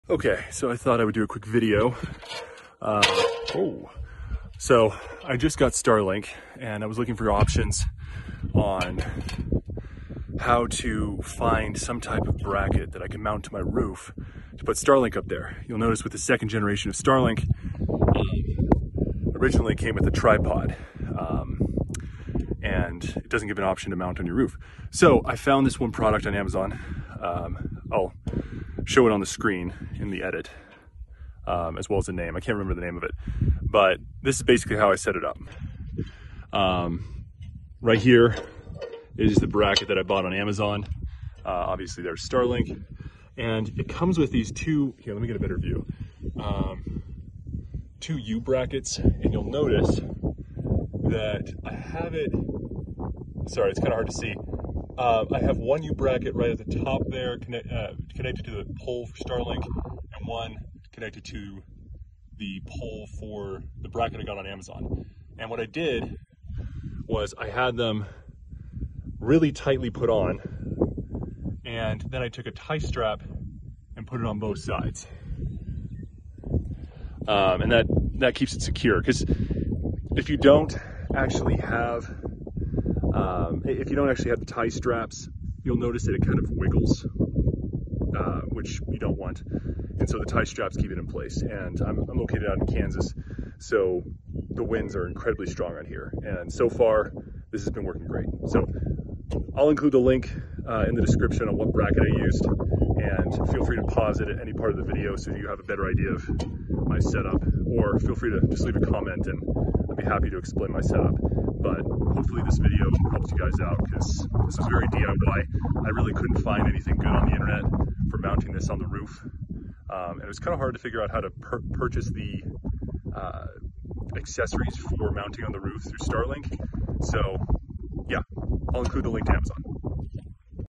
DIY Amazon Roof Mounting Bracket For Starlink 2nd Generation
DIY Amazon Roof Mounting Bracket For Starlink 2nd Generation Model.

Works great in strong kansas winds.

There are two links below of the same product. Sometimes one might be out of stock but I believe they are both the same unit so feel free to order whatever is available.

Amazon Bracket With U Links (Version 1)

Amazon Bracket With U Links (Version 2)